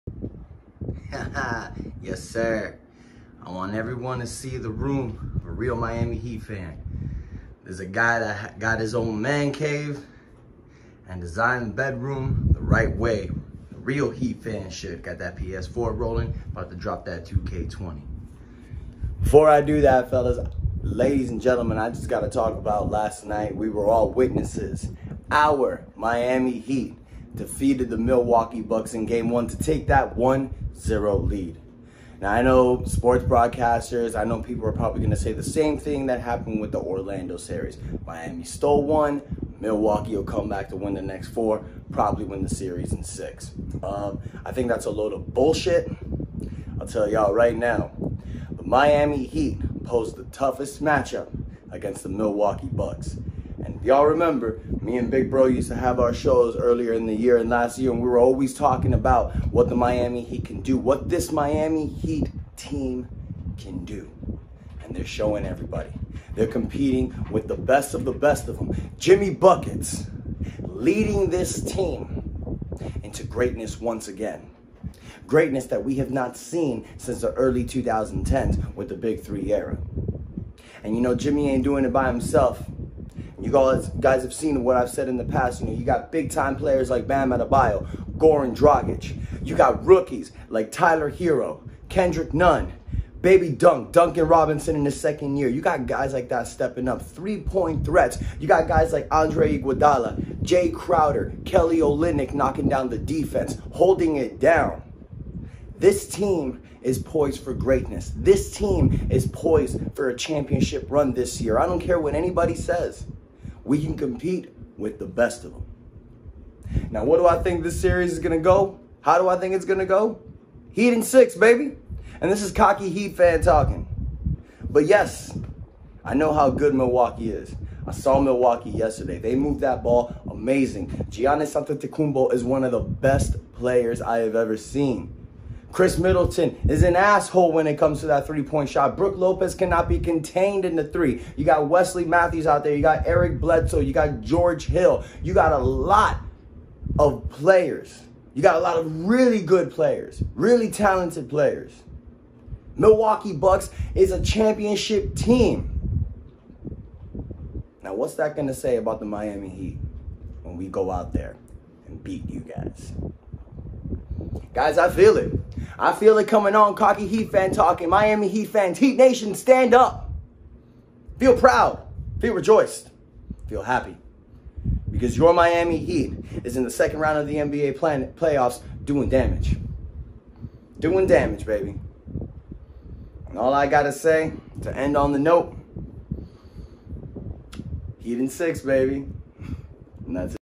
Miami Heat wins Game 1 in the NBA ECF semifinals leads series 1-0
What’s up everybody! Today is a glorious day the Miami Heat have beat Milwaukee in game one of the second round of the NBA playoffs. I will post another video breaking down an in-depth analysis. But now it’s time to celebrate